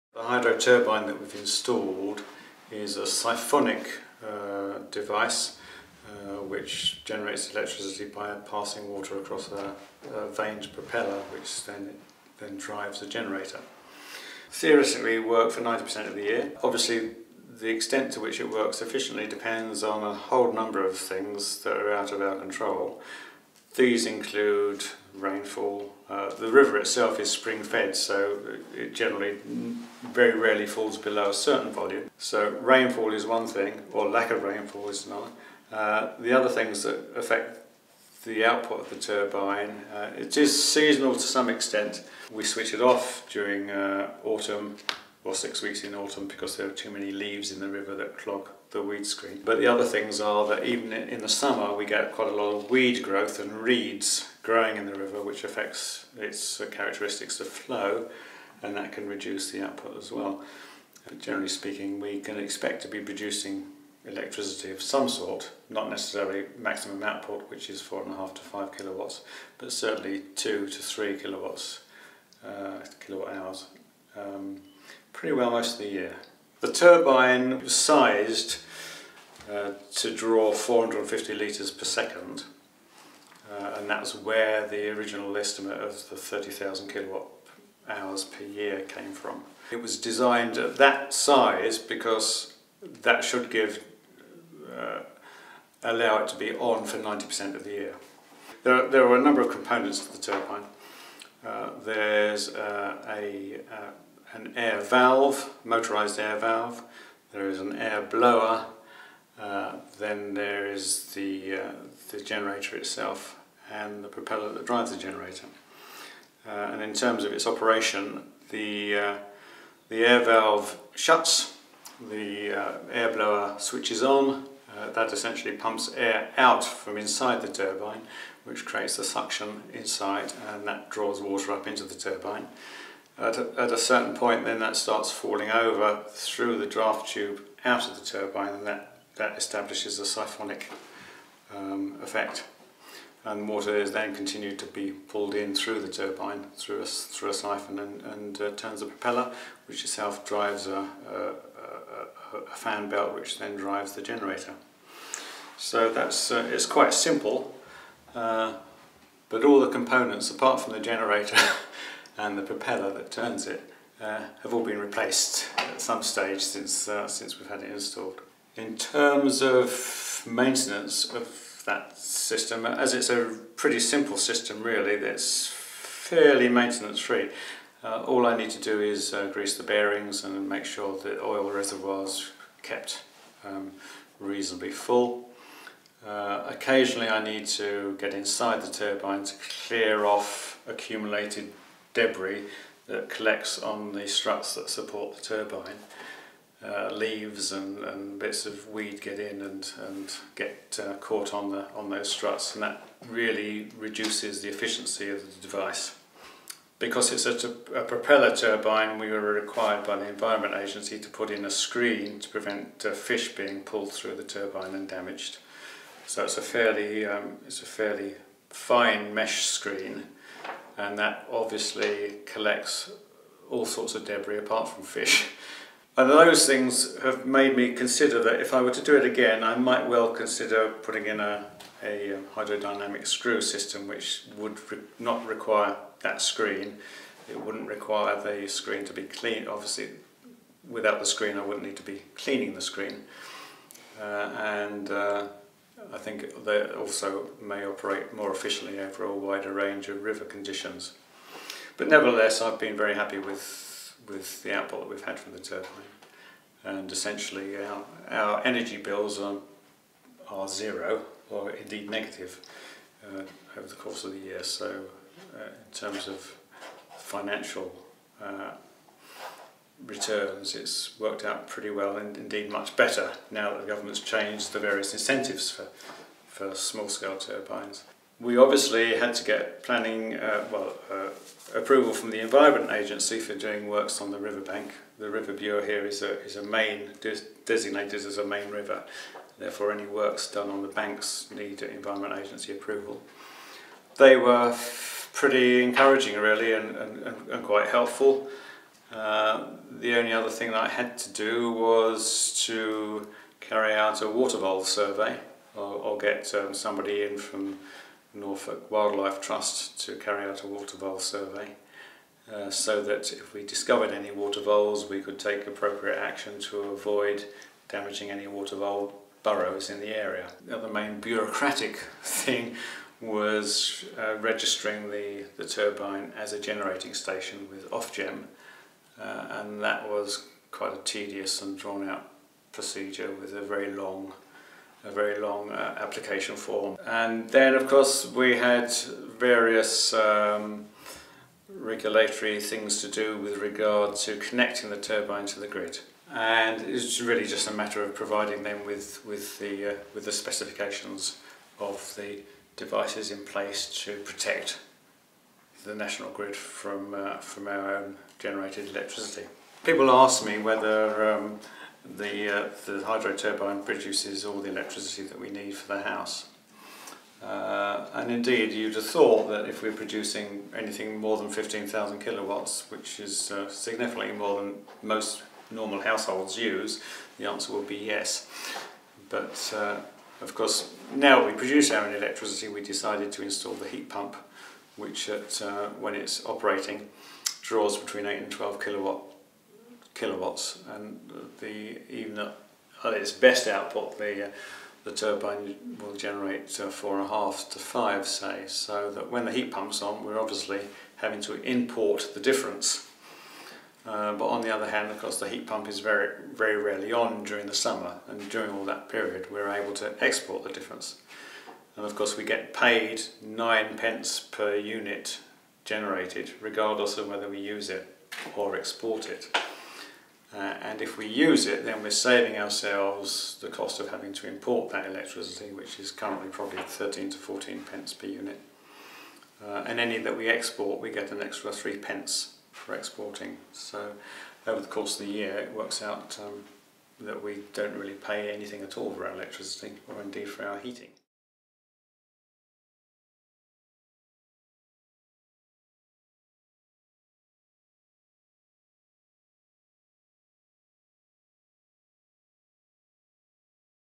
Micro Hydro at Itteringham Mill
Peter Downs uses a micro hydro turbine on the river running alongside his property to part drive a water source heat pump delivering underfloor heating in the main house. This large converted mill building from 1778 is now a guest house/B&B. In this video he explains how it works and how it has led to having zero net energy costs.

Micro hydro system
This micro hydro turbine is a siphonic device that generates electricity by passing water over a vaned propeller that then drives a generator.

Micro hydro performance
Maximum output is 4.5 - 5kW and 2-3kW is produced most of the year round. The turbine is sized to draw 450 litres per second and at this size it should be fine to have on 90% of the year. The turbine's efficiency and output depends largely on rainfall or lack of rainfall. It is switched off for 6 weeks in Autumn because too many leaves clog the weed screen. In summer an overabundance of reeds in the river can reduce the flow and also effect output.

Micro hydro components
There are a number of components in addition to the propeller and generator including a motorised air valve and air blower.

How the turbine works
The air valve shuts, the air blower switches on pumping air out from inside the cylindrical turbine housing. This creates the suction inside which draws water up into the turbine. At a certain point the water starts falling over through the draft tube out of the turbine. This establishes the siphonic effect so that water continues to be pulled through the turbine and over the propeller which turns a fan belt which turns the generator.

Micro hydro maintenance
The micro hydro turbine does not require very regular maintenance. You need to grease the bearings and make sure the oil reservoir is kept reasonably full. Occasionally you also need to get inside the turbine and clear leaves and weeds other debris from the struts that support the turbine. That said most of the components apart from the generator and the propeller on this micro hydro turbine have been replaced at some stage.

Protecting fish
A fine mesh screen was installed to prevent fish from getting caught in the turbine but this collects all sorts of debris other than fish. If Peter were to do it again, he'd consider a hydrodynamic screw system which wouldn't require the screen, would do away with the cleaning and might work better over a variety of river conditions.

Permissions for micro hydro
Approval from the Environment Agency is required for carrying out any works on the river banks. They were encouraging and quite helpful in this case. They requested that a water vole survey be carried out so action to avoid any water vole burrows could be taken.

The turbine had to be registered as a generating station with Ofgem which was quite a drawn out procedure with a long application form. Then there were some regulatory requirements to do with connecting the turbine to the grid. They needed to know the spec of the turbine to ensure it was a suitable feed for the national grid.

Feed-in tariffs for micro hydro
Thanks to feed in tariffs 9p is earned per unit generated by the turbine. When used, the generated electricity also saves importing grid electricity as a cost of some 13-14p a unit (green tariff). When exporting 3p per unit is earned on top of the payment for generating green electricity.

Return on Investment
The turbine provides all the electricity for the house in summer plus some for export to the grid, but in winter it is providing electricity for a water source heat pump which draws between 8 - 12kW. The turbine, at its best, provides 4- 5kW so the difference must be imported in winter. However, at the end of the year it works out that electricity and heating is being provided at no cost. Energy bills are negative, so the turbine has worked out well in terms of financial returns.

SuperHome
Peter Down's home has undergone extensive eco renovation and includes solar thermal panels, replica double glazed sash and casement windows, and extensive insulation to loft, roof and floor. It is one of the homes in the SuperHomes network of older homes that have been renovated by their owners to reduce carbon emissions by at least 60%. Most SuperHomes host public open days in spring or September. For more, visit: